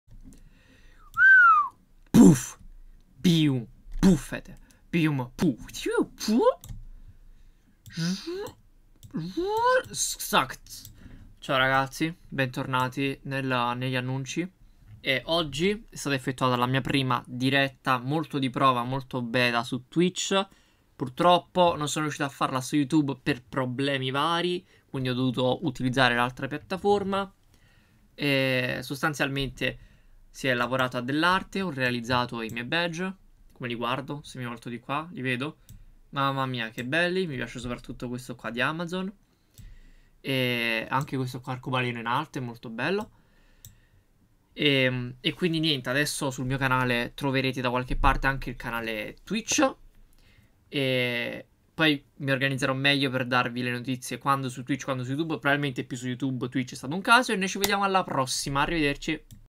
Esito prima Live: Approdo anche su Twitch!
gg per me che non sono stato capace di salvarla in tempo twich infame che rende l'opzione nascosta youtube infame che mi deve far aspettare giorni.